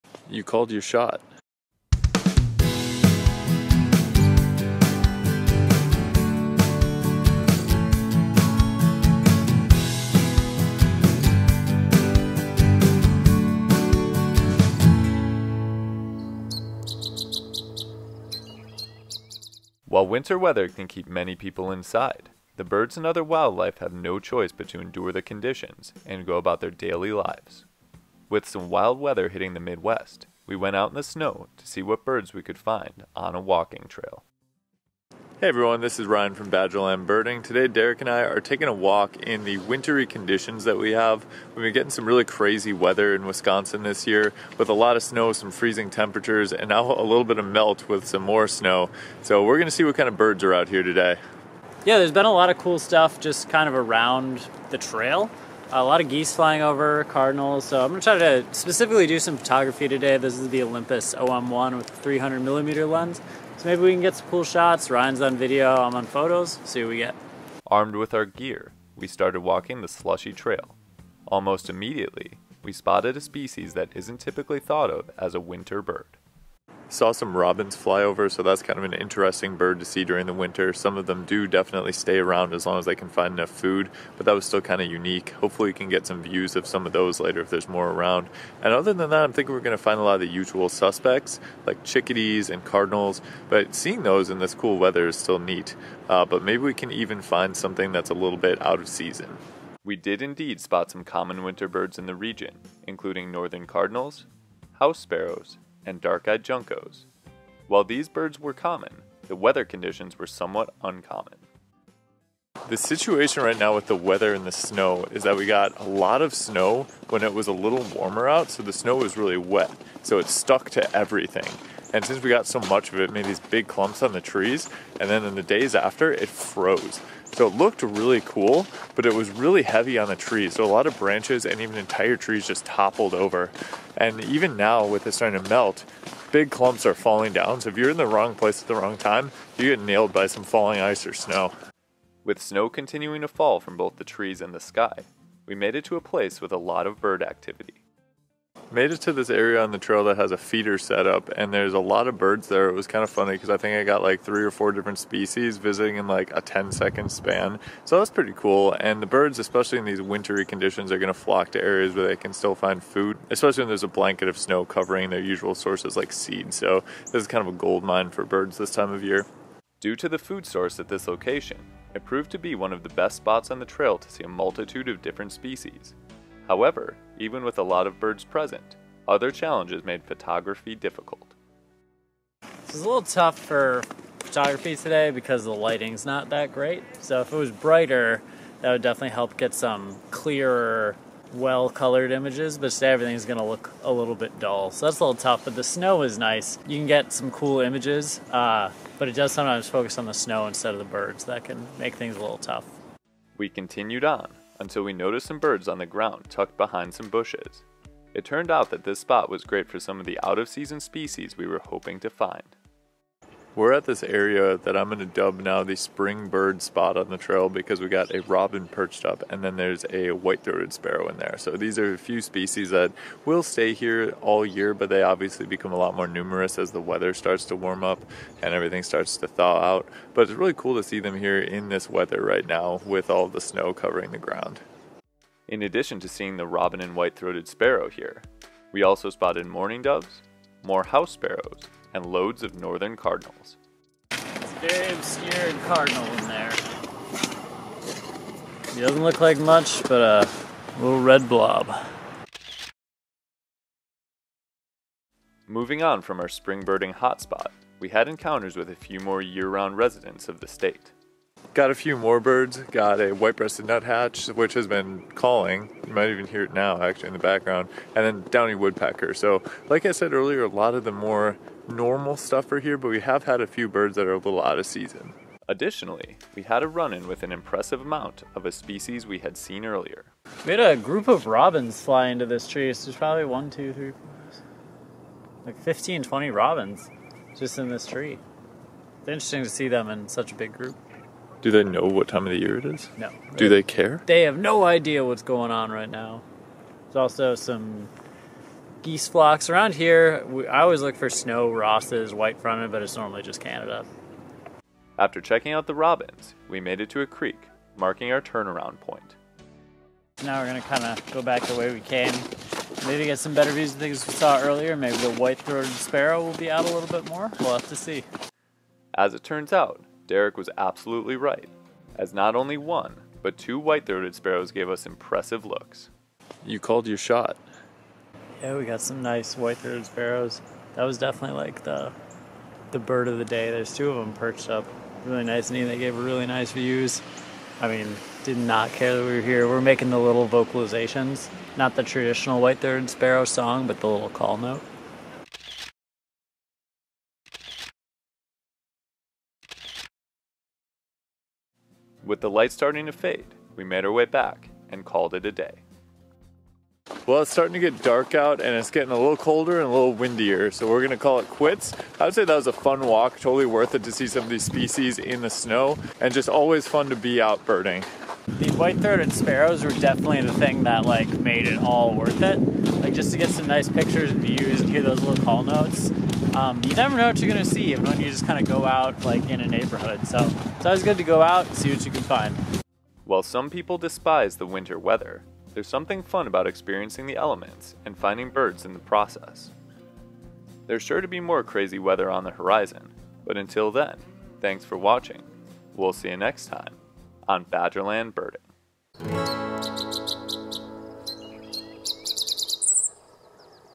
Birding and Bird Photography in a Beautiful Snowy Landscape
Winter in the northern parts of the continental United States offer unique opportunities and challenges during the winter. Join Ryan and Derek from Badgerland Birding on a quest to record and photograph birds on a snowy winter day in Wisconsin.

Nikon D5300
Derek's vlogging and macro camera